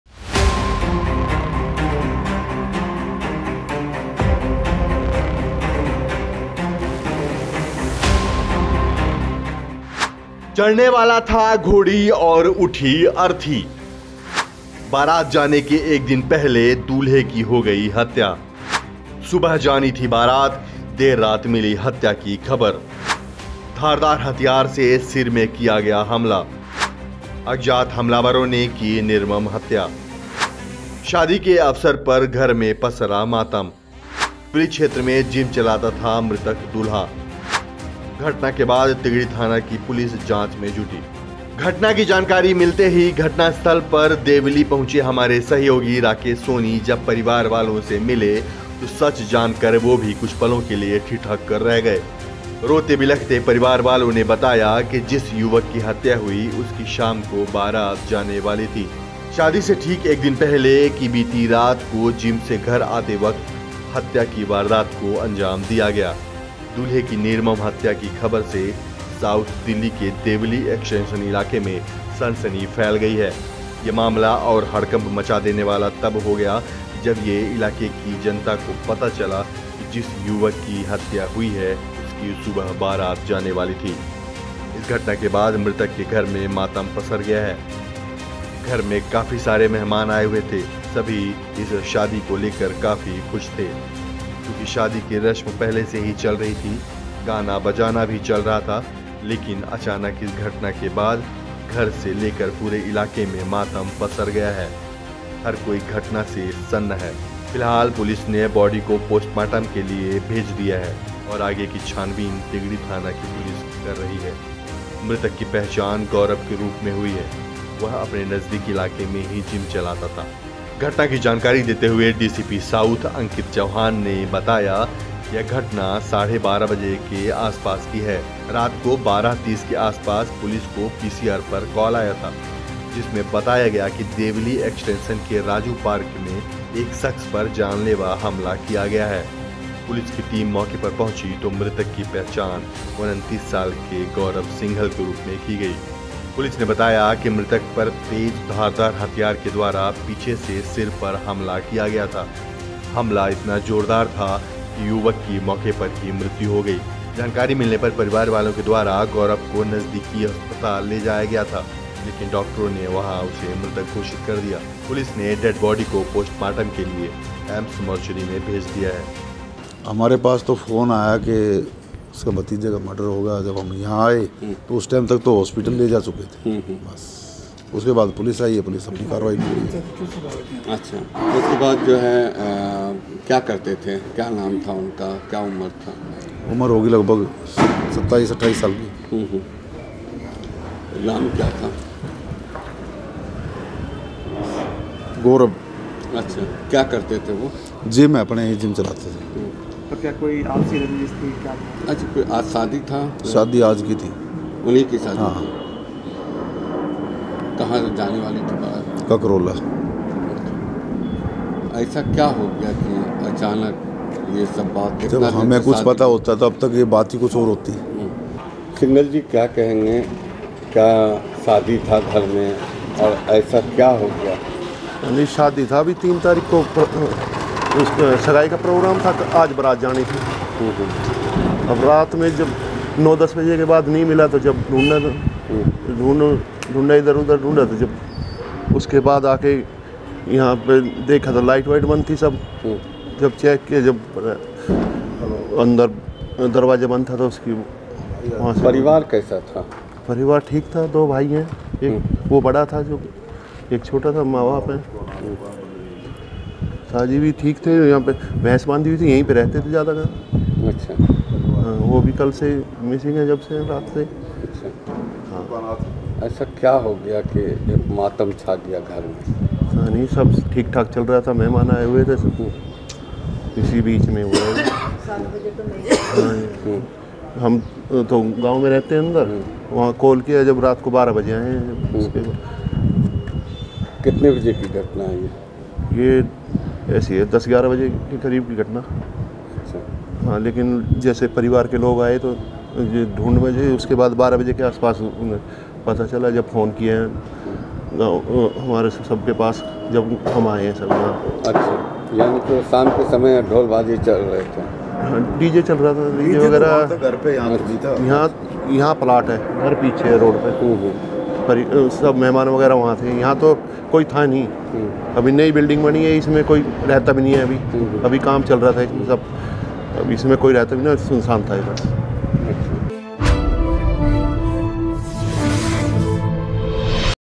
Crime News : शादी की एक रात पहले दूल्हे की अज्ञात हमलावरों ने धारदार weapon से किया Murdered
शार्प हथियार से उसपर वार किया गया था। उसे तुरंत मैक्स हॉस्पिटल में परिवार वालों के द्वारा ले जाया गया था  लेकिन डॉक्टर ने वहां उसे मृत घोषित कर दिया। उसके बाद डेड बॉडी को पोस्टमार्टम के लिए एम्स मोर्चरी में भेज दिया गया है। इस घटना के बाद घर मे मातम पसर गया है. घर मे काफी सारे मेहमान आये हुए थे. सभी इस शादी को लेकर काफी खुश थे चुकि शादी की रश्म पहले से हीं चल रही थी. गाना बजाना भी चल रहा था. लेकिन अचानक इस घटना के बाद घर से लेकर पुरे इलाके मे मातम का माहौल हो गया है. हर कोई इस घटना से सन्न है. फिलहाल पुलिस बॉडी को पोस्टमार्टम के लिए भेज दिया है औऱ  आगे की छानबीन तिगड़ी थाना की पुलिस टीम के द्वारा की जा रही है।